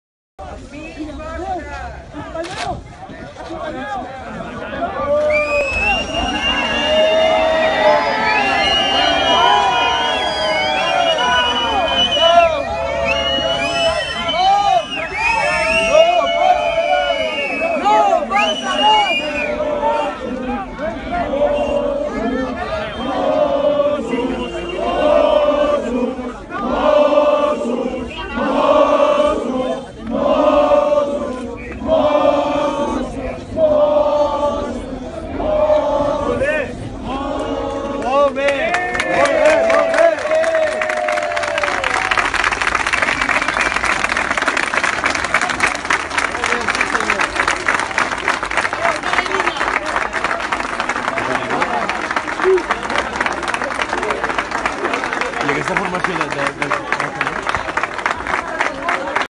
Catalan Police steps back for Protesters -REFERENDUM-
Catalan riot police steps back to let protesters protest for independe,it is well known that catalan police mossos d'esquadra are helping the protesters to gain independence